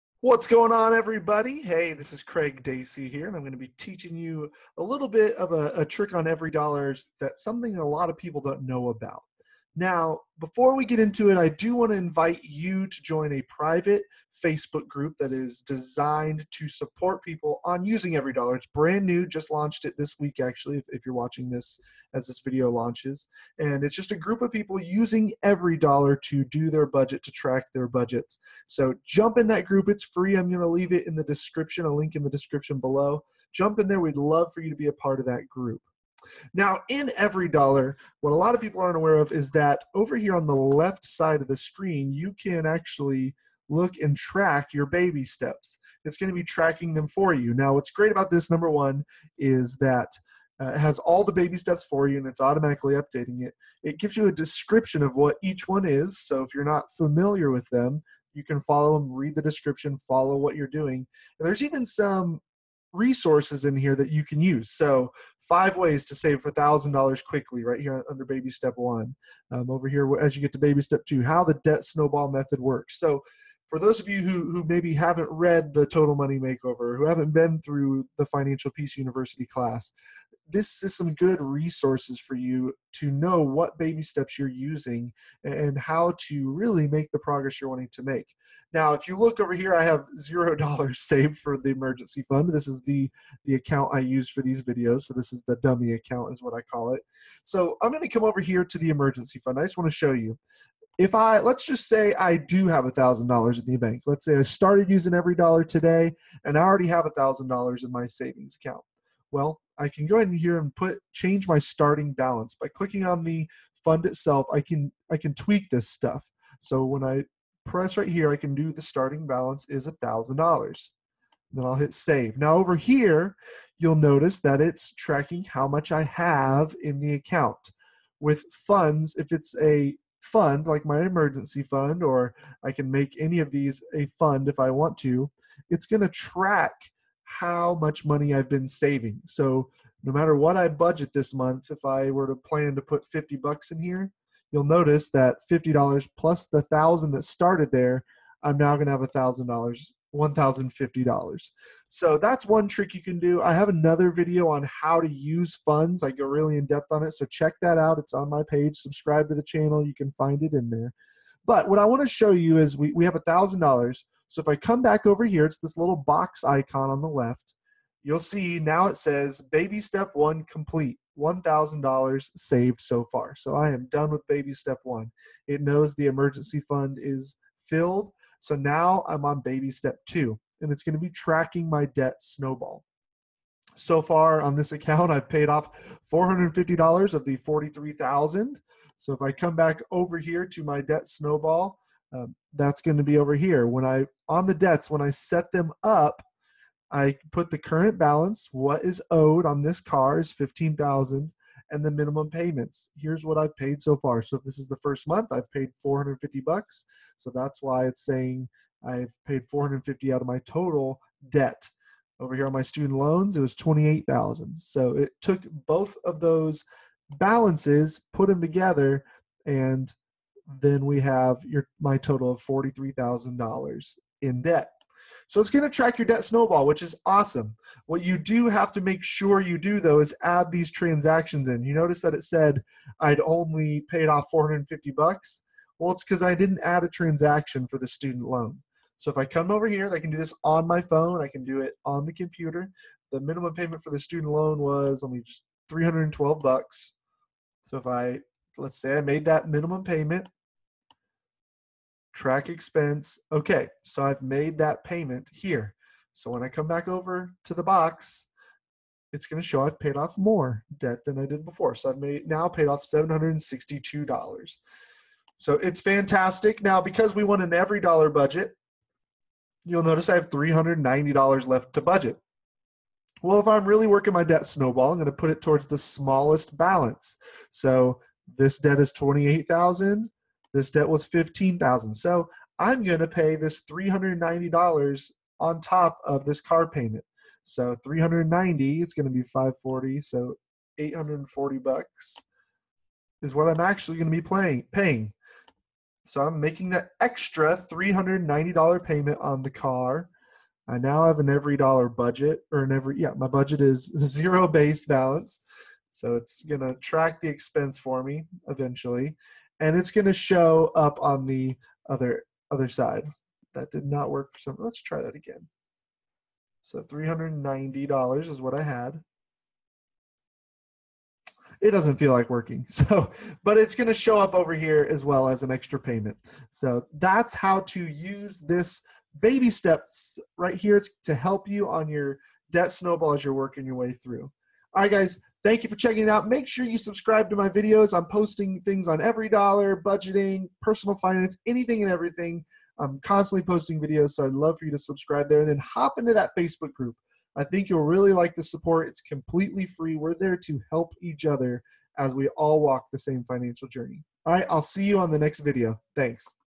EveryDollar Tutorial: Tracking Your Baby Steps
Did you know that you can track your Baby Steps inside of EveryDollar? Check out this unknown secret within the EveryDollar website!